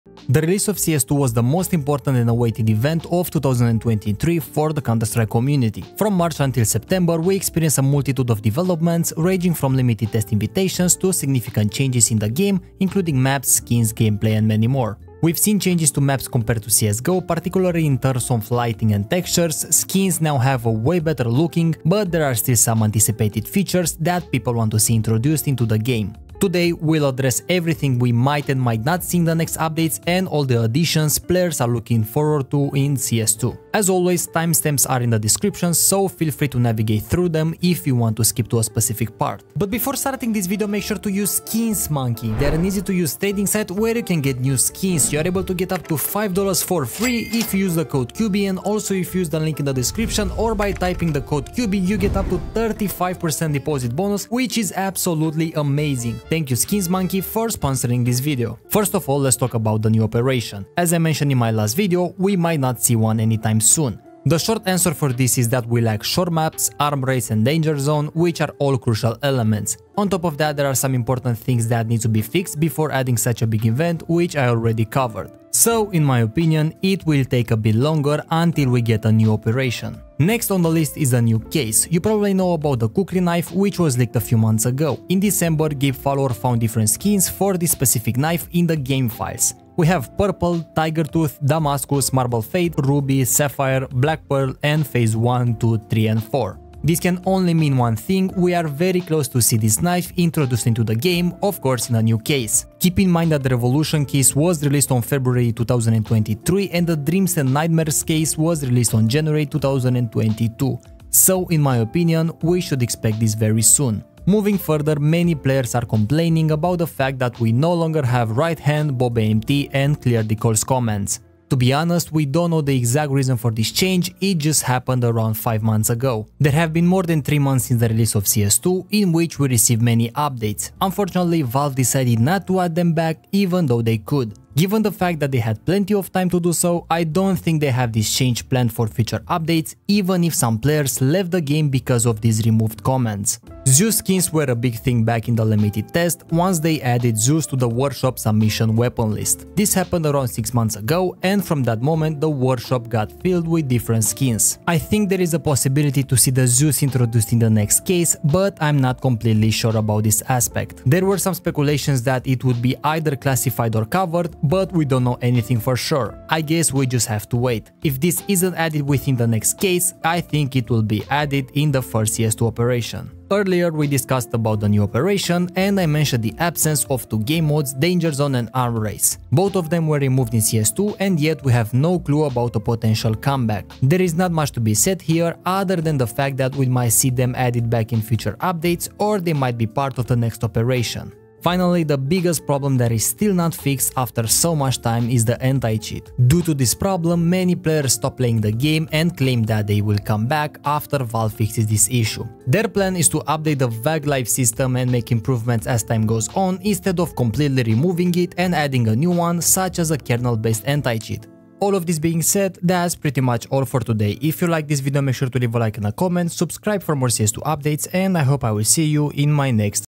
CS2 Next Update? Operation / Case / Knife
In today's video we will see what could be added in the next CS2 update, from a new operation to the only command that the community wants back, cl_righthand 1

0:00 - Intro
0:58 - New Operation
1:20 - New Case
1:54 - Right Hand & Bobamt
2:24 - Zeus
2:53 - Danger Zone & Arms Race
3:11 - Anti-Cheat